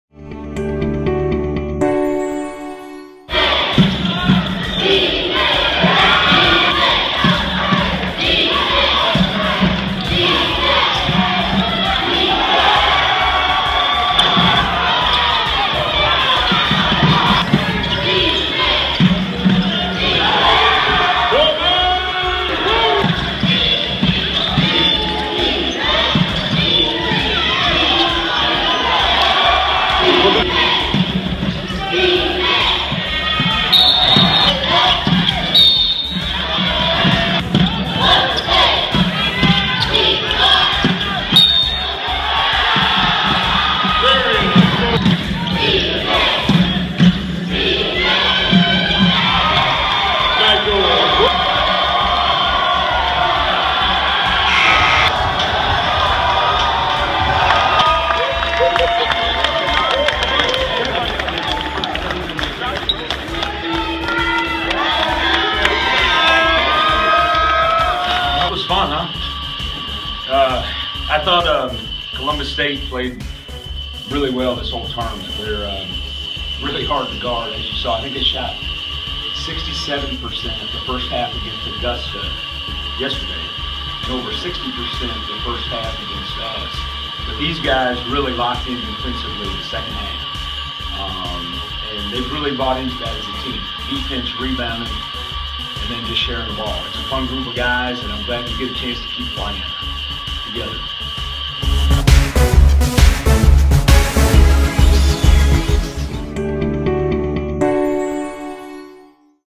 UNCP 2017 Peach Belt Conference tournament champs
UNCP defeated Columbus State 89-77 to win the PBC tournament title.